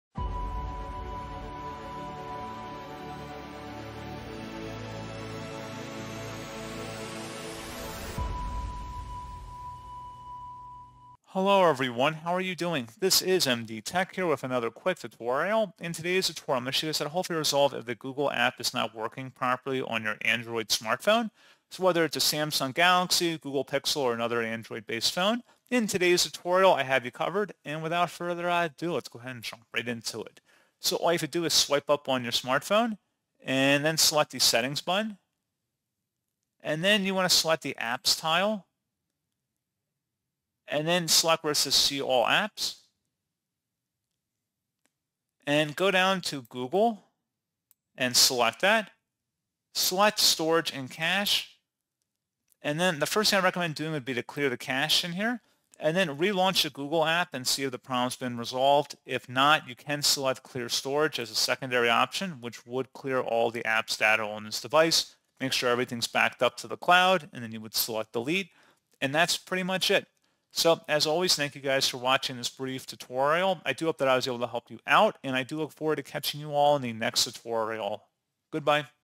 How To Fix Google App Not Working On Android Phone [2026]
How To Fix Google App Not Working On Android Phone [Guide]

This guide provides effective solutions to fix the issue of the Google app not working on Android phones. Users can troubleshoot common problems like the app freezing, crashing, or failing to load by clearing the cache, checking for updates, or adjusting app permissions. The tutorial also covers methods such as restarting the device or reinstalling the Google app to restore functionality. By following these steps, users can quickly resolve issues and get the Google app working properly again on their Android phone.

Issues addressed in this tutorial:
google app not working on android
why is my google not working on my android phone